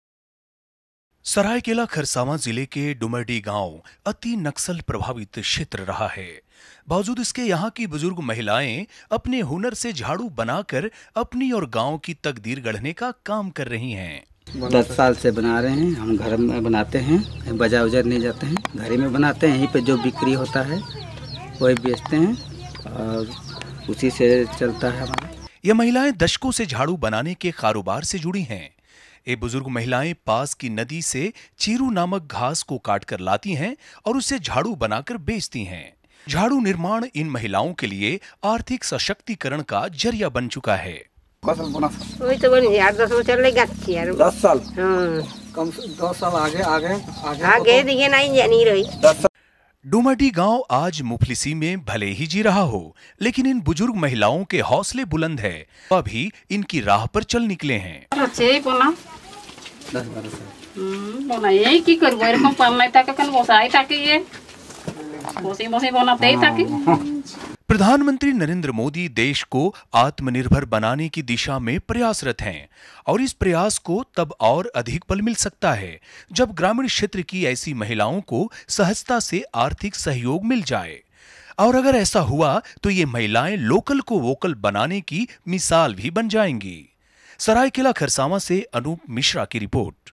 sarikela 15
सरायकेला_खरसावां जिले के ईचागढ़ प्रखंड के डुमरडीह गांव में महिलायें भी आत्मनिर्भरता की मिसाल पेश कर रही हैं। महिलायें अपने हुनर से झाड़ू बनाकर अपनी और गांव की तकदीर गढ़ने का कार्य कर रही हैं